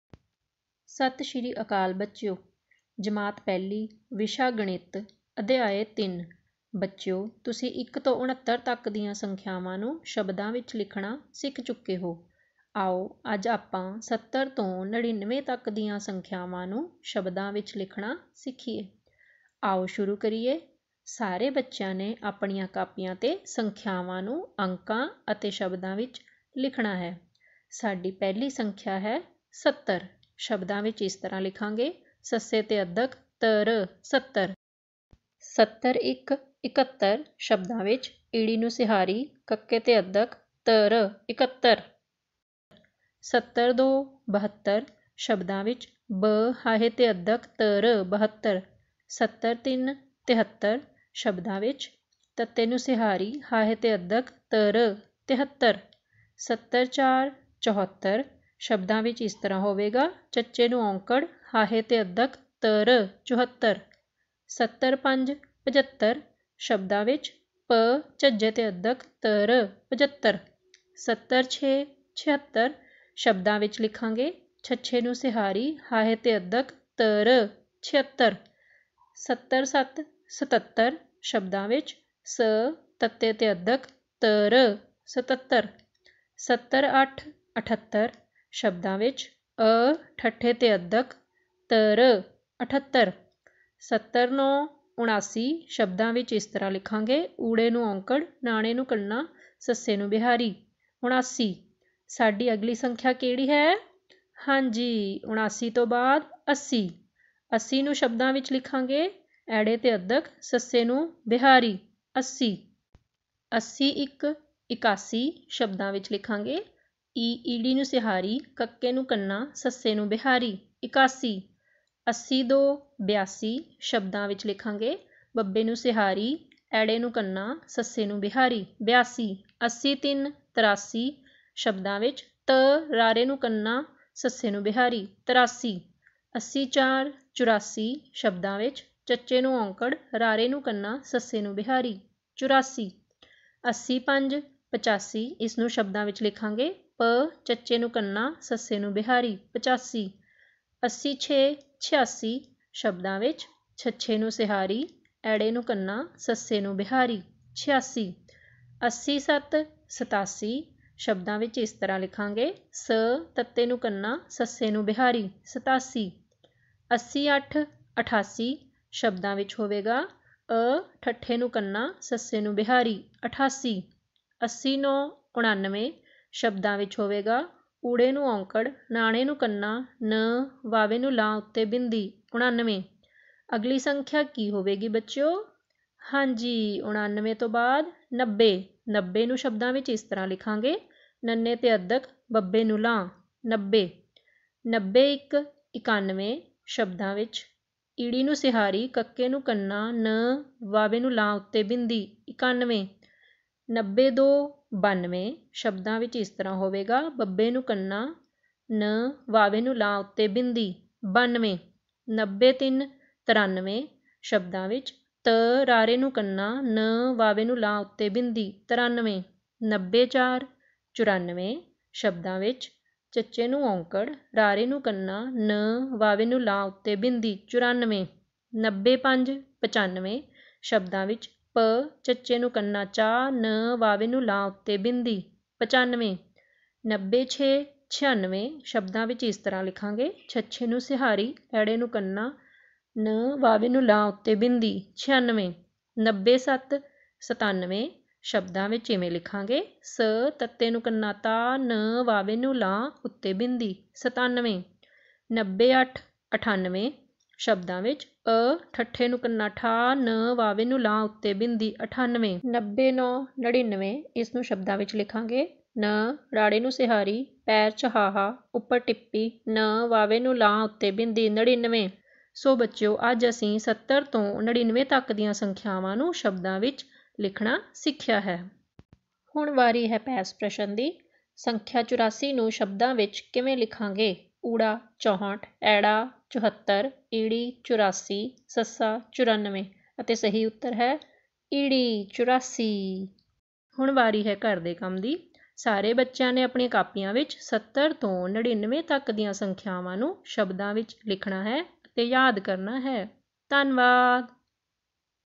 Learn to write 70 to 99 in figure and in words in Punjabi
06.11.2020

ਜਮਾਤ : ਪਹਿਲੀ (PSEB)

ਗਣਿਤ  ਦੀ ਦੁਨੀਆ
ਅਧਿਆਇ : 3

70 ਤੋਂ 99 ਤੱਕ ਅੰਕਾਂ ਨੂੰ ਸ਼ਬਦਾਂ ਵਿੱਚ ਲਿਖਣਾ ਸਿੱਖਣਾ ।